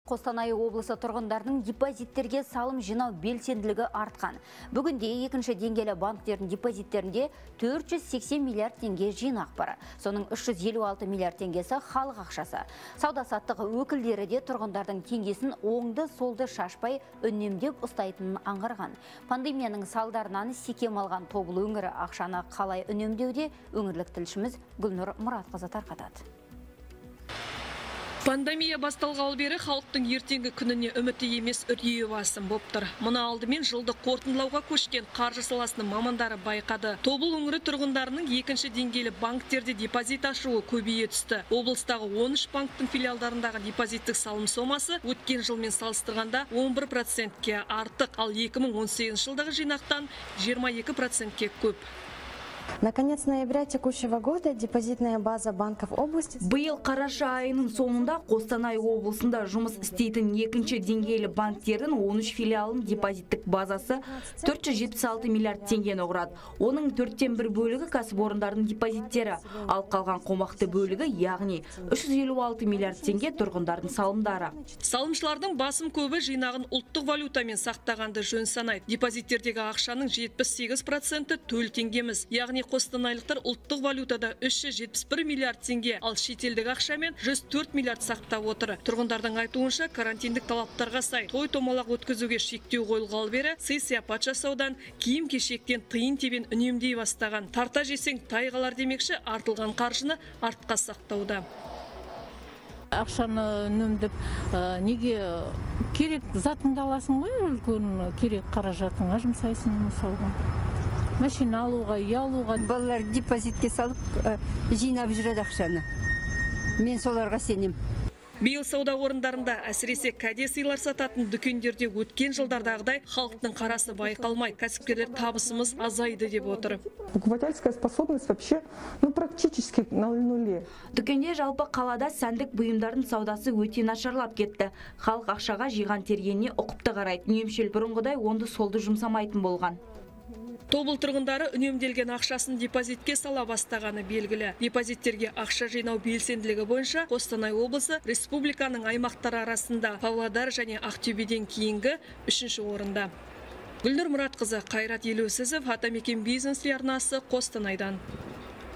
ТОБЫЛ ӨҢІРІ ТҰРҒЫНДАРЫНЫҢ ДЕПОЗИТТЕРГЕ САЛЫМ ЖИНАУ БЕЛСЕНДІЛІГІ АРТҚАН
Қостанай облысы тұрғындарының депозиттерге салым жинау белсенділігі артқан. Бүгінде екінші деңгейлі бакнтердің депозиттерінде 480 млрд теңге жинақ бар. Соның 356 млрд теңгесі халық ақшасы. Сауда-саттық өкілдері де  тұрғындардың теңгесін оңды-солды шашпай, үнемдеп ұстанайтынын аңғарған.  Пандемияның салдарынан секем алған Тобыл өңірі  ақшаны қалай үнемдеуде? Бұл туралы өңірлік тілшіміз тарқатады.